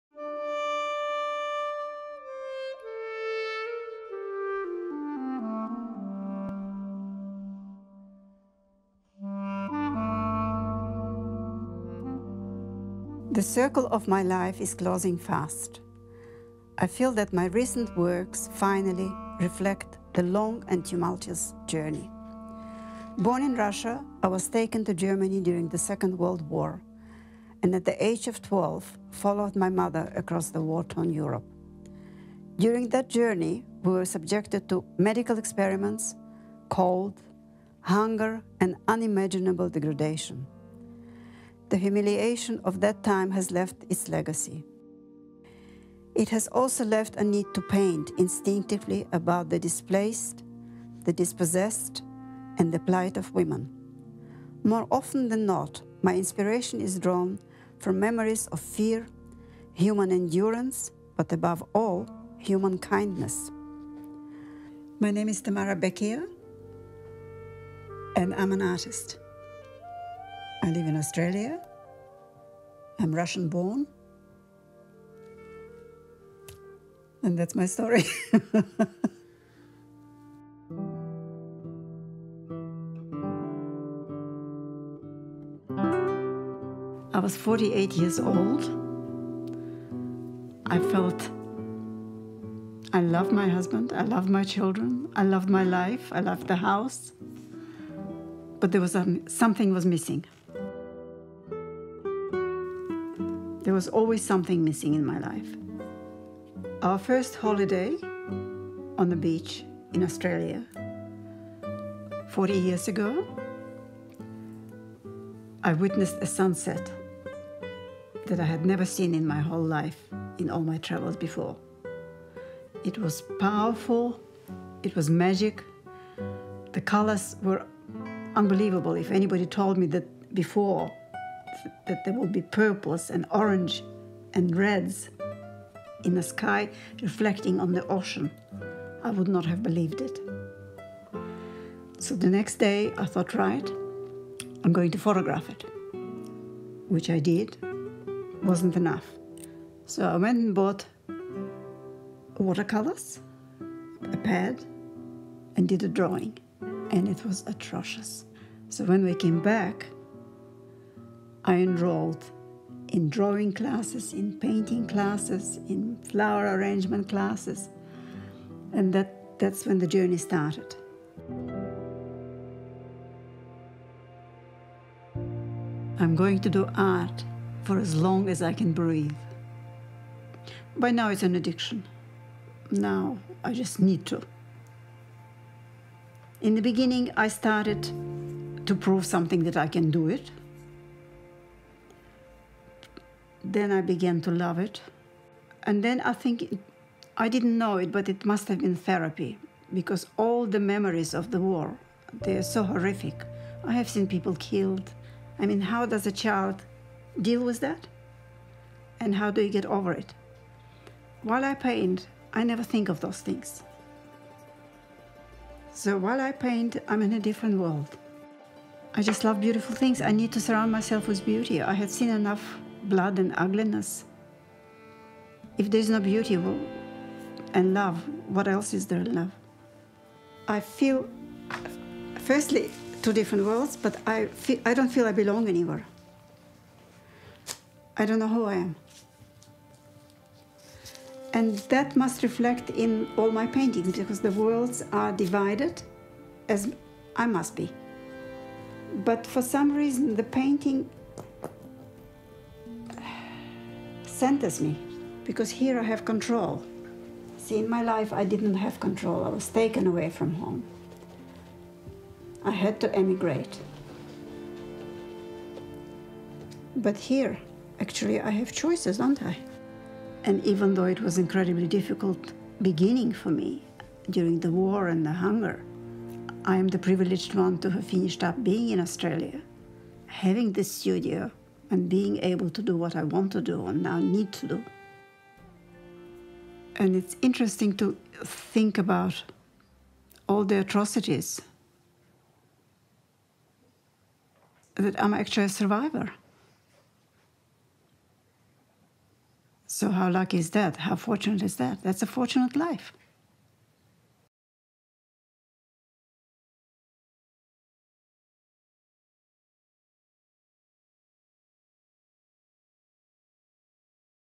Art ‘therapy’ for WWII survivor Tamara Bekier | Art Works
In this story we meet Tamara Bekier, whose art has enabled her to make meaning from the enormity of her life experiences.

Born in Russia, Tamara was taken to Germany during World War II and subsequently followed her mother across a devastated Europe. At just 12 years of age Tamara, like so many, suffered unimaginable trauma, and the humiliation has left a lasting legacy.

Through her art, which she took up after her first trip to Australia, Tamara began to find the missing piece in her life.

Tamara’s inspiration is drawn from memories of fear, human endurance and above all, human kindness.